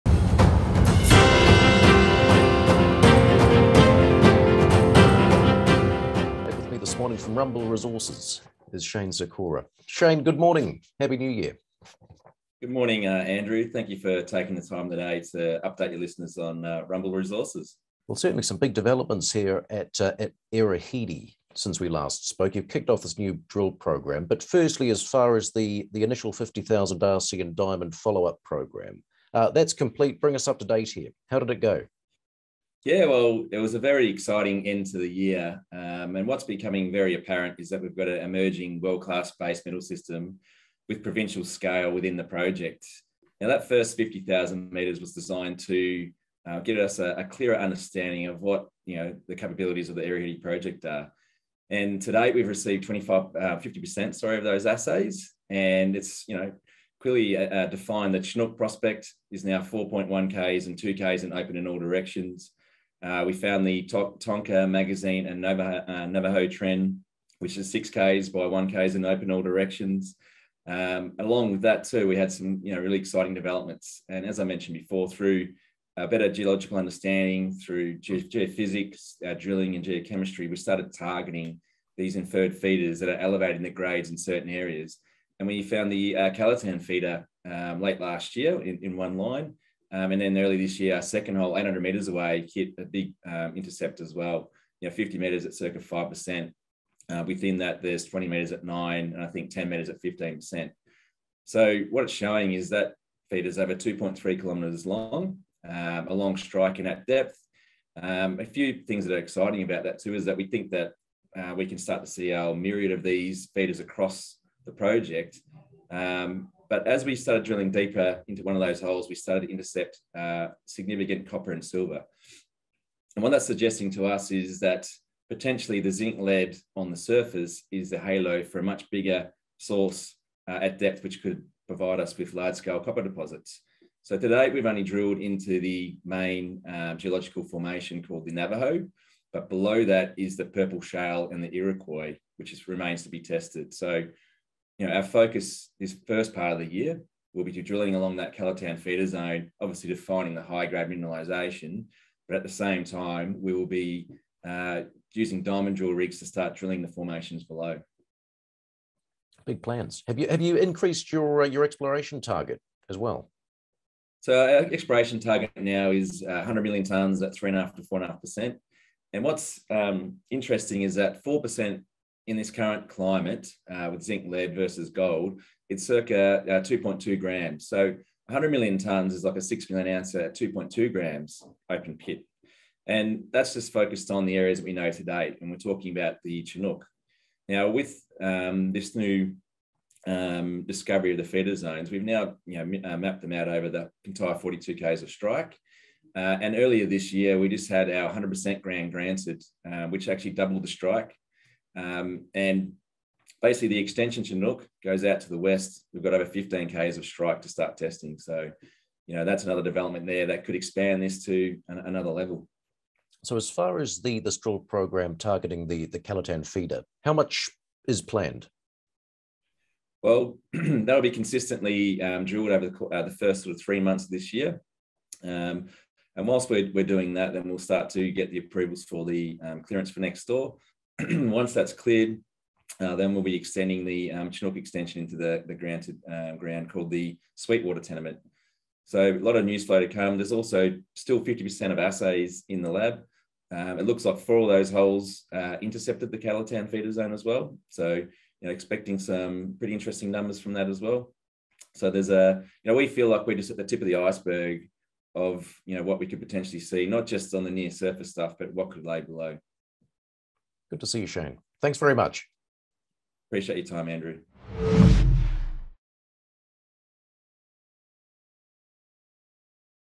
Rumble Resources confident of world-class base metals at Kalitan Feeder Zone as it starts drilling
Shane Sikora tells Proactive about the Kalitan Feeder Zone at the Earaheedy project in Western Australia, where the company is busy with a multi-rig drill program to extend the resource, and there are high hopes for a world-class base metals system of provincial scale.